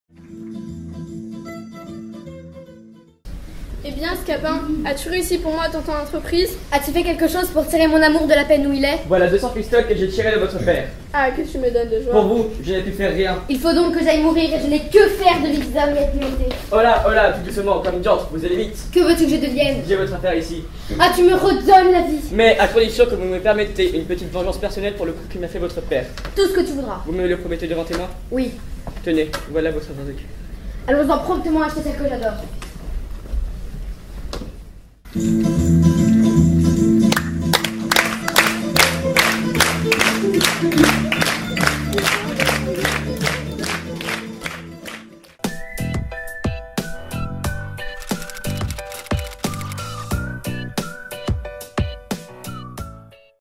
Les Fourberies de Scapin - Acte II Scène 8 - InterCycles
Pièce de théâtre de Molière, en texte quasi-intégral, jouée par des élèves de 5ème et des jeunes du Centre d'Education Motrice de Dommartin.